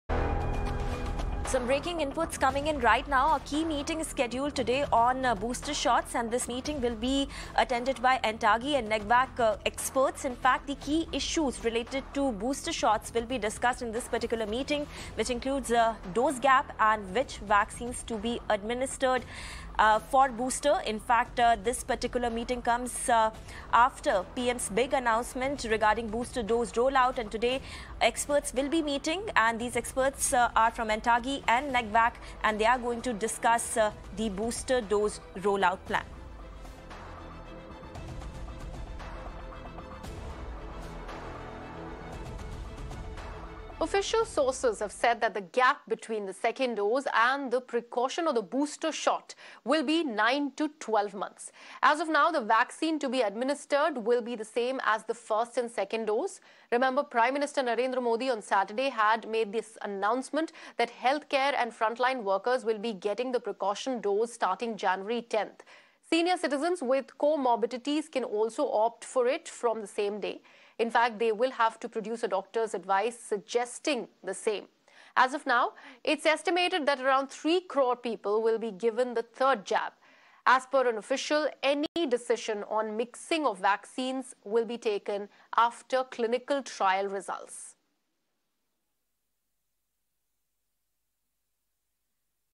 Booster Shot In India: NTAGI & NEGVAC Key Meeting On Booster Shot Administration Today
After Prime Minister’s Modi announcement on Covid booster shot administration, a key meeting to discuss the rollout is scheduled for today. Official sources say that the gap between the second dose and the precaution or booster shot will be 9-12 months. As of now, the vaccine to be administered will be the same as the first and second doses. Amid the Omicron scare in India, the government has announced vaccines for children and booster doses for frontline workers and senior citizens with morbidities of the country. The rollout is scheduled to begin in January 2022. Watch the video to know more.